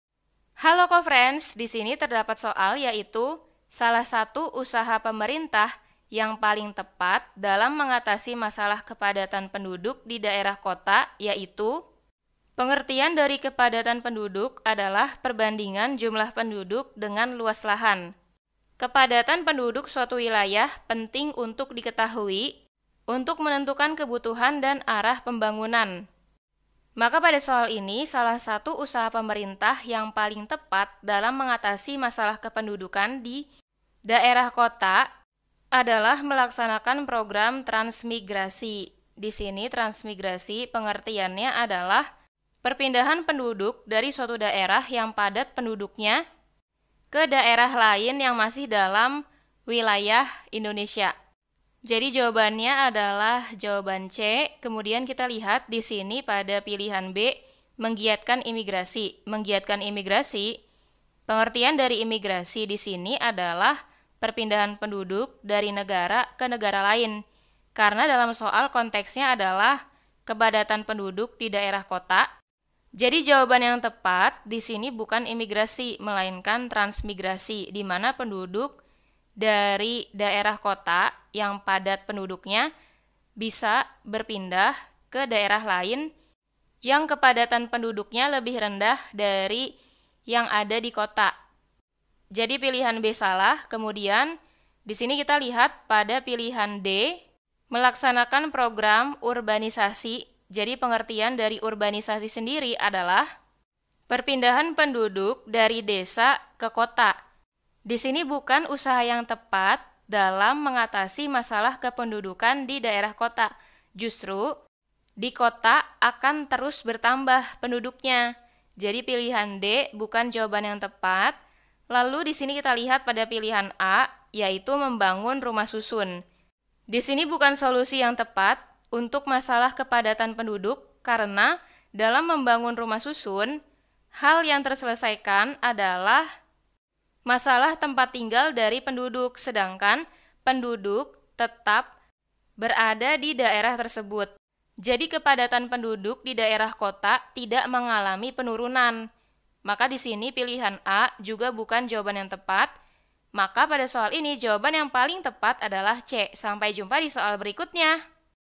Salah satu usaha pemerintah yang paling tepat dalam mengatasi masalah kepadatan penduduk di daera...
Salah satu usaha pemerintah yang paling tepat dalam mengatasi masalah kepadatan penduduk di daerah kota, yaitu .

Topik atau Materi: Pencemaran Lingkungan - Fisika Geografi - Fisika Lingkungan - Fisika Kelas 7 SMP -

Sumber Soal: Fisika SMP SMA aqilacourse.net , N/A

00:00 : Bedah Soal
01:00 : Konsep, rumus dan pengertian Pencemaran Lingkungan
01:18 : Langkah penyelesaian soal
01:38 : Jawaban akhir
02:08 : Penutup